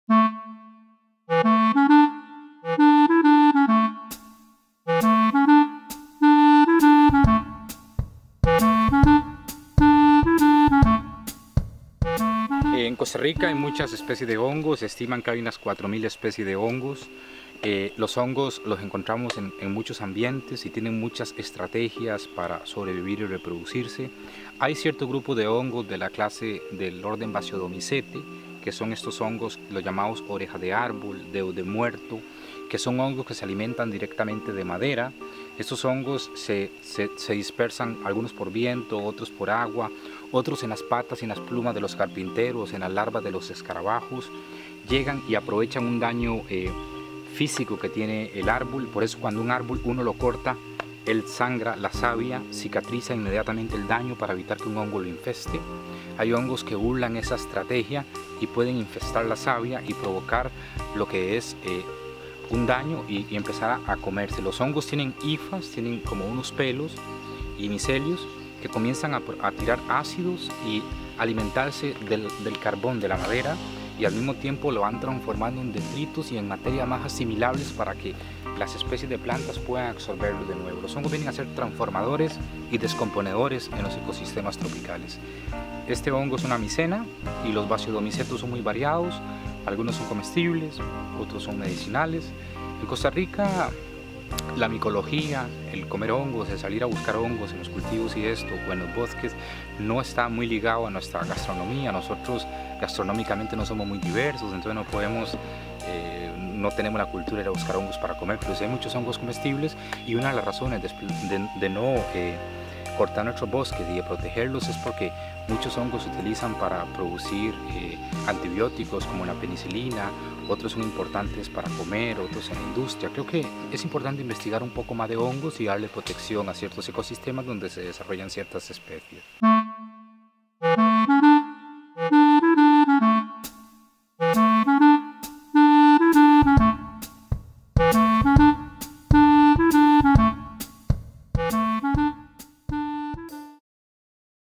Bosque & Ciencia
Ulises nos habla de los hongos  y su función dentro del bosque.